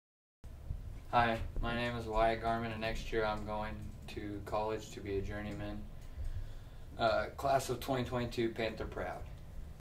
Wyatt Garman Panther Proud
CHS is proud of our seniors! Here is another video in our series that let's our seniors tell you where they are headed next year. Class of '22, Panther Proud!